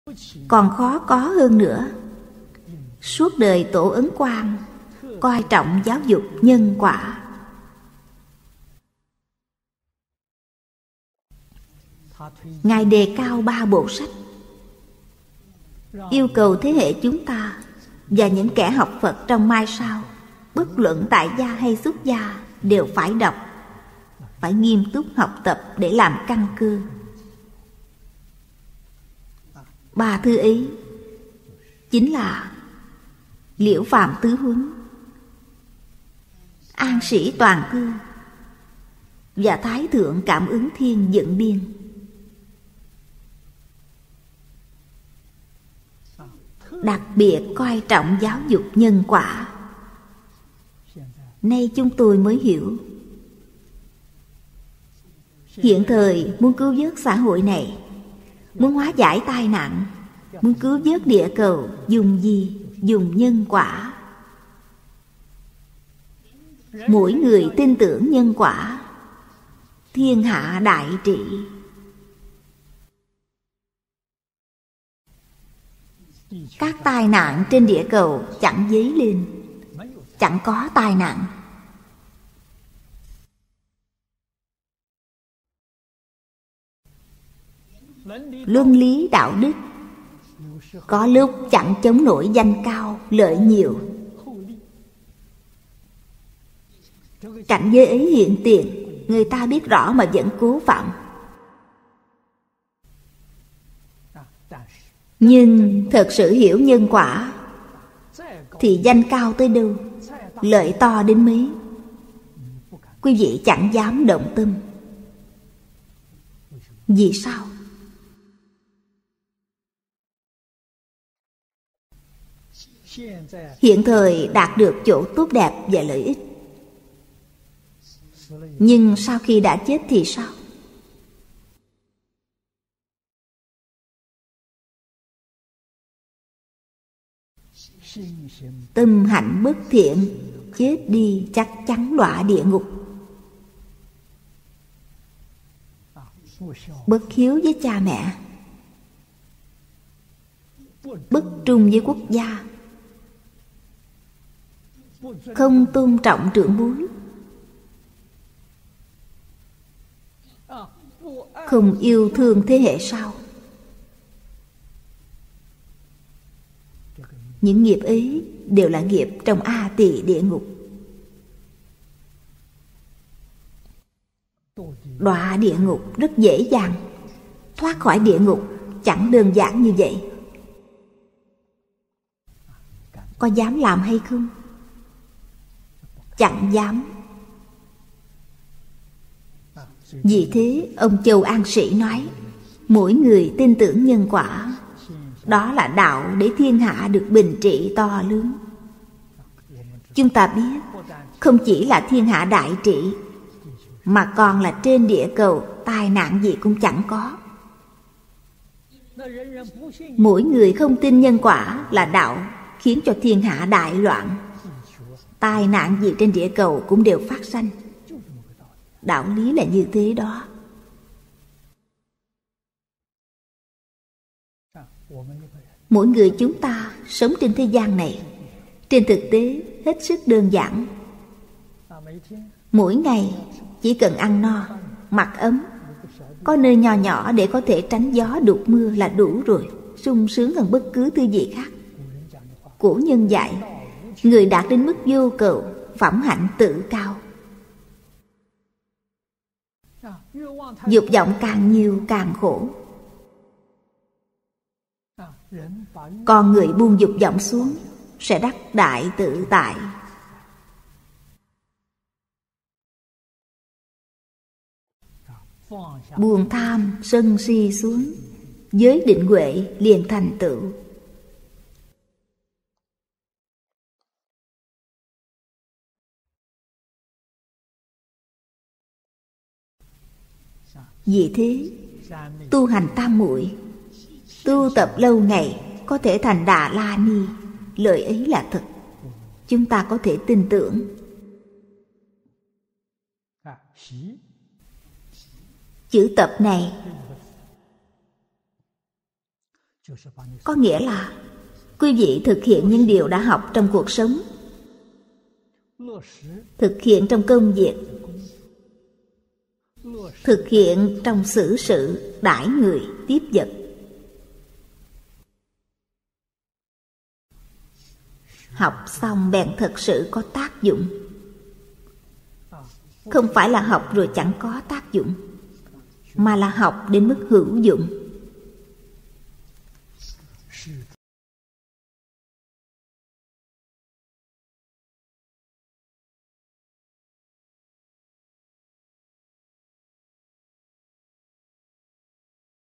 TĐ:1238- Cả đời tổ Ấn Quang, ngài đặc biệt coi trọng giáo dục nhân quả
Tịnh Độ Đại Kinh Giải Diễn Nghĩa:
Thời gian từ: 00h56m33:03 – 01h02m03:13
{Có thể chặn quảng cáo video - ngay cả trên Facebook và YouTube}

Còn khó có hơn nữa, suốt đời tổ Ấn Quang coi trọng giáo dục nhân quả. Ngài đề cao ba bộ sách, yêu cầu thế hệ chúng ta và những kẻ học Phật trong mai sau, bất luận tại gia hay xuất gia đều phải đọc, phải nghiêm túc học tập để làm căn cơ. Ba thứ ấy chính là Liễu Phàm Tứ Huấn, An Sĩ Toàn Thư, và Thái Thượng Cảm Ứng Thiên Vựng Biên, đặc biệt coi trọng giáo dục nhân quả. Nay chúng tôi mới hiểu: Hiện thời, muốn cứu vớt xã hội này, muốn hóa giải tai nạn, muốn cứu vớt địa cầu, dùng gì? Dùng nhân quả. Mỗi người tin tưởng nhân quả, thiên hạ đại trị, các tai nạn trên địa cầu chẳng dấy lên, chẳng có tai nạn. Luân lý, đạo đức có lúc chẳng chống nổi danh cao, lợi nhiều, cảnh giới ấy hiện tiền, người ta biết rõ mà vẫn cố phạm. Nhưng thật sự hiểu nhân quả thì danh cao tới đâu, lợi to đến mấy, quý vị chẳng dám động tâm, vì sao? Hiện thời đạt được chỗ tốt đẹp và lợi ích, nhưng sau khi đã chết thì sao? Tâm hạnh bất thiện, chết đi, chắc chắn đọa địa ngục. Bất hiếu với cha mẹ, bất trung với quốc gia, không tôn trọng trưởng bối, không yêu thương thế hệ sau, những nghiệp ấy đều là nghiệp trong A Tỳ địa ngục. Đọa địa ngục rất dễ dàng, thoát khỏi địa ngục chẳng đơn giản như vậy, có dám làm hay không? Chẳng dám! Vì thế, ông Châu An Sĩ nói: “Mỗi người tin tưởng nhân quả, đó là đạo để thiên hạ được bình trị to lớn”. Chúng ta biết, không chỉ là thiên hạ đại trị, mà còn là trên địa cầu, tai nạn gì cũng chẳng có! “Mỗi người không tin nhân quả là đạo khiến cho thiên hạ đại loạn”, tai nạn gì trên địa cầu cũng đều phát sanh, đạo lý là như thế đó! Mỗi người chúng ta sống trên thế gian này, trên thực tế, hết sức đơn giản, mỗi ngày chỉ cần ăn no, mặc ấm, có nơi nho nhỏ để có thể tránh gió đụt mưa là đủ rồi, sung sướng hơn bất cứ gì khác. Cổ nhân dạy: “Người đạt đến mức vô cầu, phẩm hạnh tự cao”. Dục vọng càng nhiều càng khổ, con người buông dục vọng xuống sẽ đắc đại tự tại. Buông tham, sân, si xuống, Giới, Định, Huệ liền thành tựu. Vì thế, “tu hành tam-muội, tu tập lâu ngày có thể thành Đà-la-ni”, lời ấy là thật, chúng ta có thể tin tưởng. Chữ Tập (習) này có nghĩa là quý vị thực hiện những điều đã học trong cuộc sống, thực hiện trong công việc, thực hiện trong xử sự, đãi người, tiếp vật, học xong bèn thật sự có tác dụng, không phải là học rồi chẳng có tác dụng, mà là học đến mức hữu dụng.